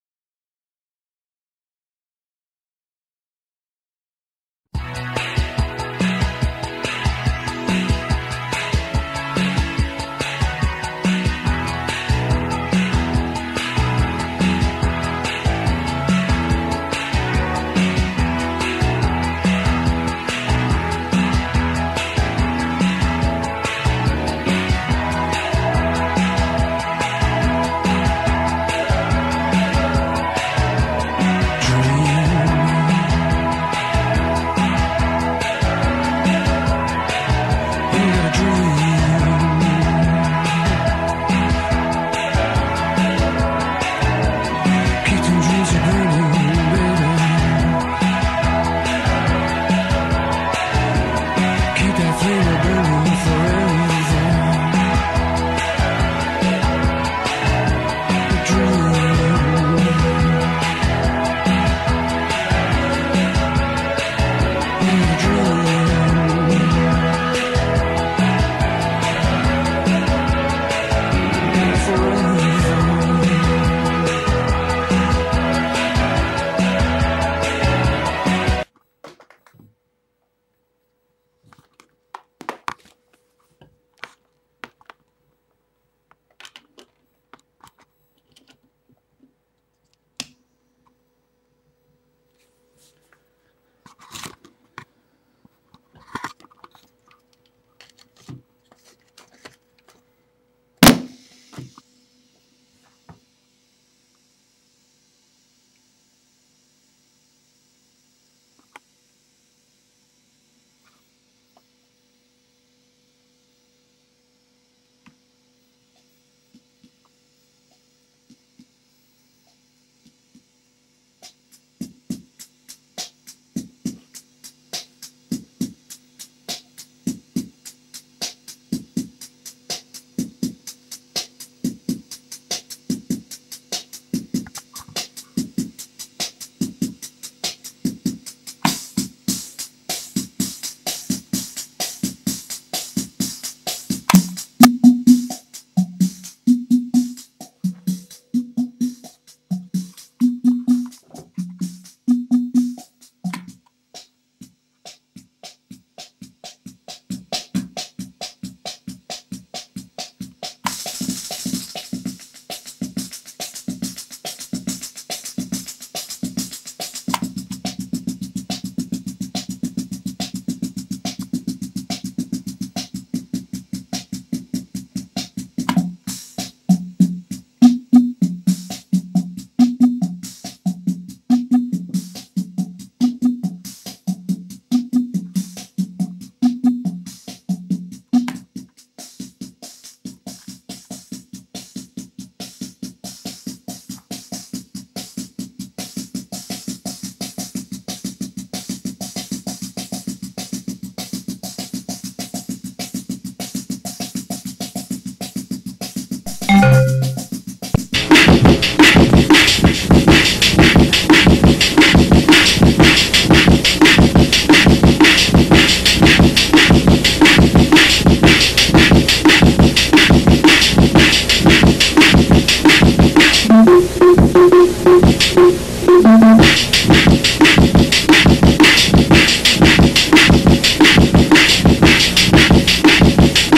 Build your own drum machine.
I put this retro-sounding drum machine together for less than $100. Took a bit a stuffing around, was pretty complicated, but well worth it. Have used it at gigs and for recordings. It's a little dangerous and has electrocuted me a couple times, so be very careful if you try this.
Thoughts and comments are appreciated. :)